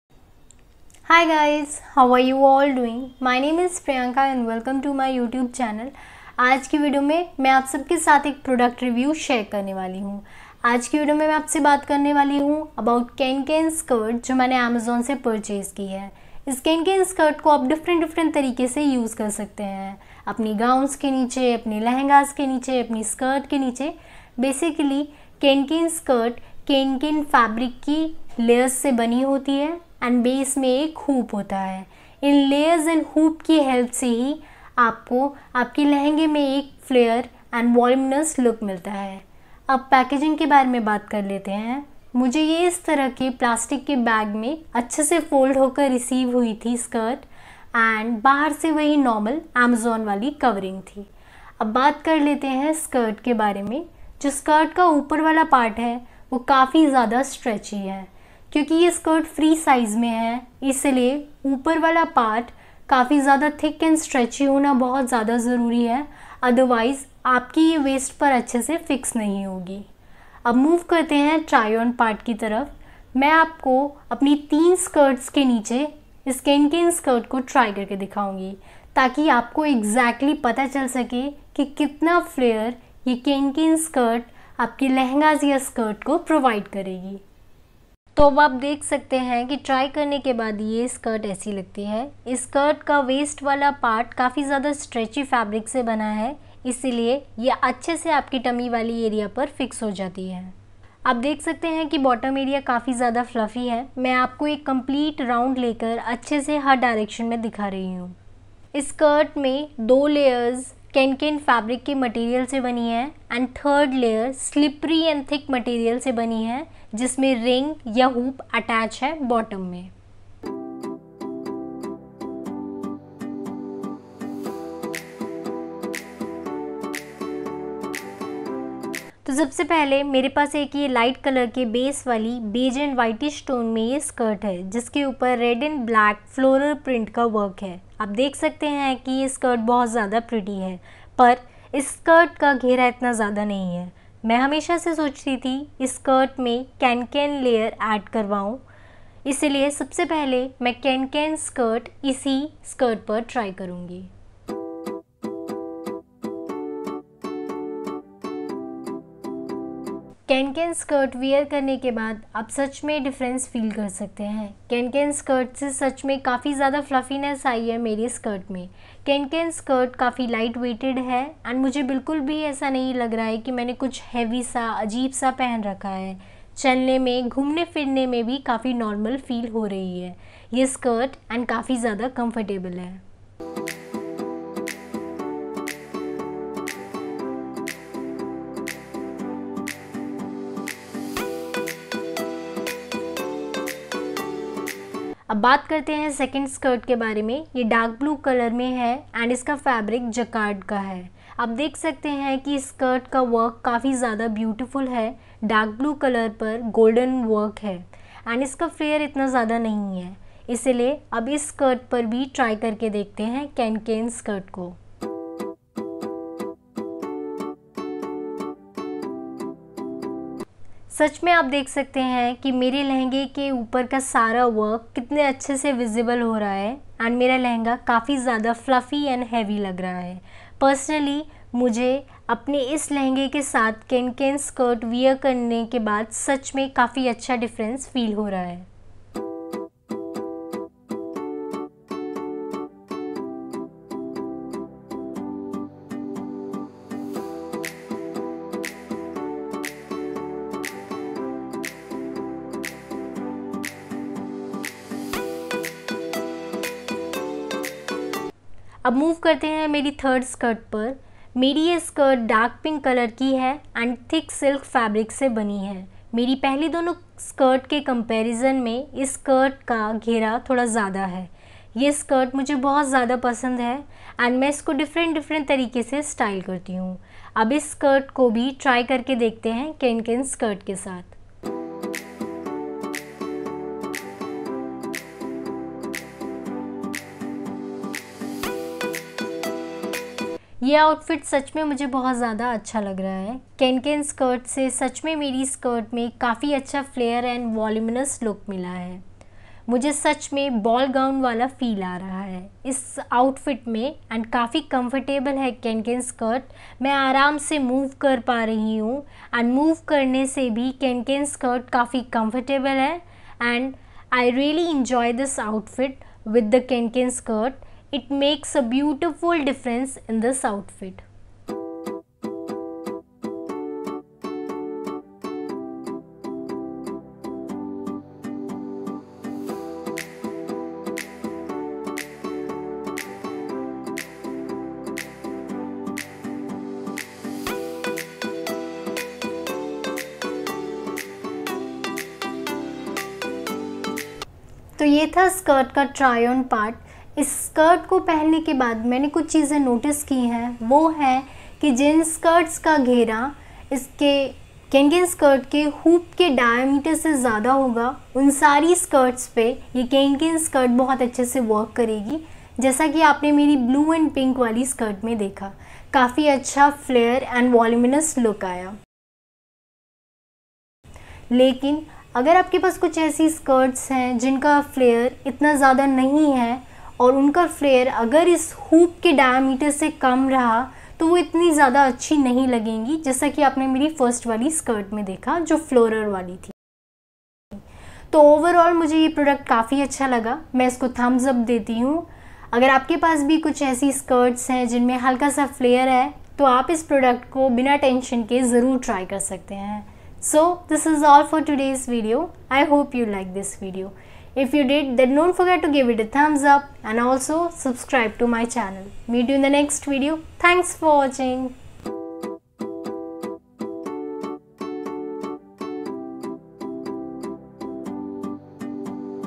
Amazon Cancan Skirt /Petticoat Review with Demo | Kya cancan skirt lehenge ko volume deta hai ?
Hii Guys!!
Today's video is all about cancan skirt review from Amazon/restyle lehenga, long skirts, gown in weddings with cancan/reuse old clothes with
cancan skirt
cancan skirt review
cancan petticoat

black bralette